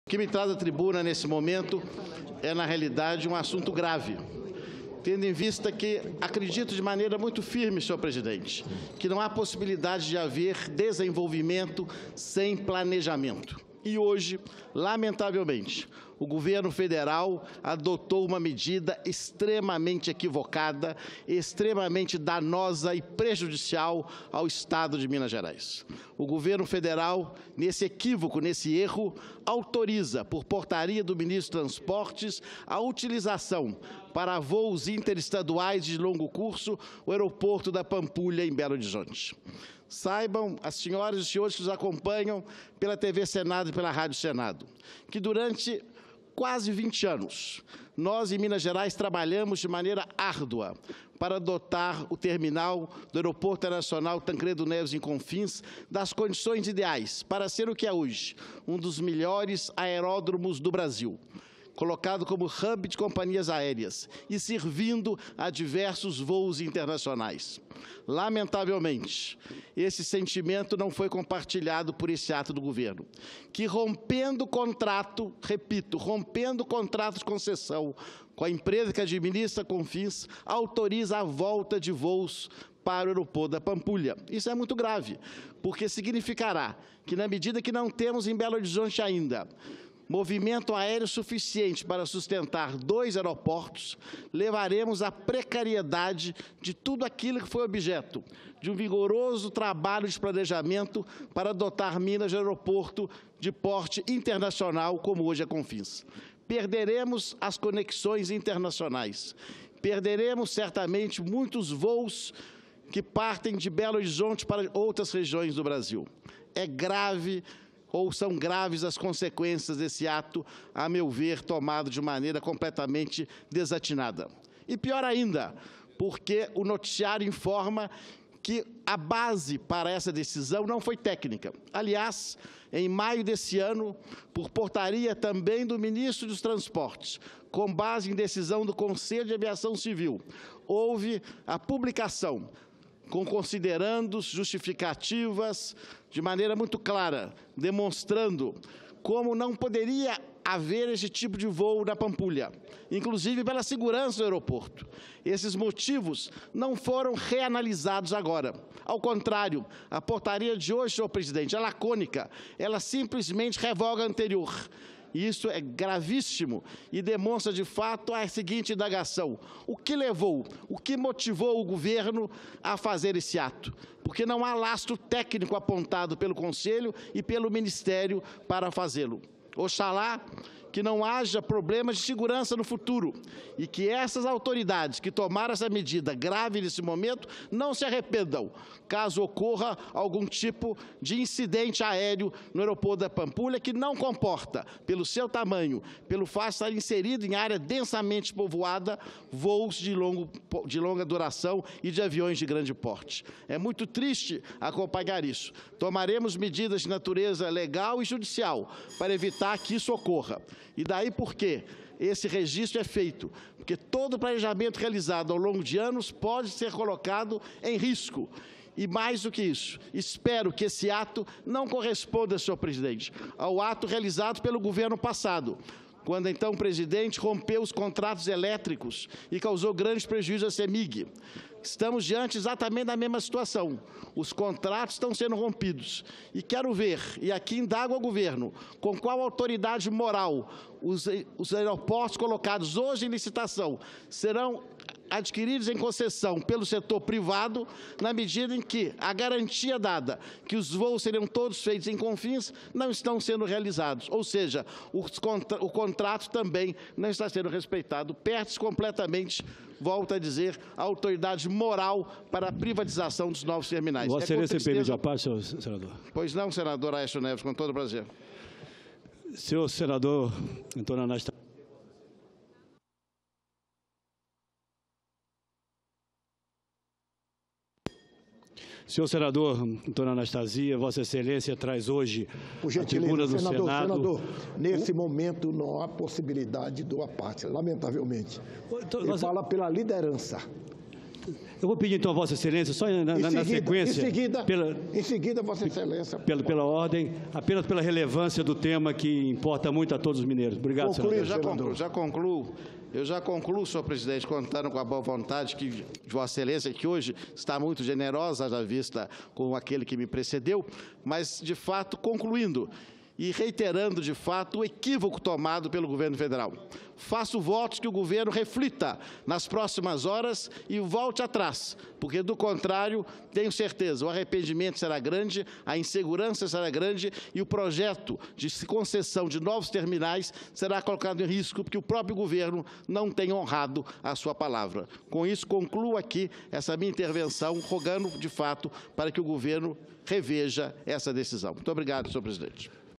Anastasia alerta sobre portaria que autoriza voos de longo curso para o Aeroporto da Pampulha
25/10/2017 - Plenário Discursos - Anastasia alerta sobre portaria que autoriza voos de longo curso para o Aeroporto da Pampulha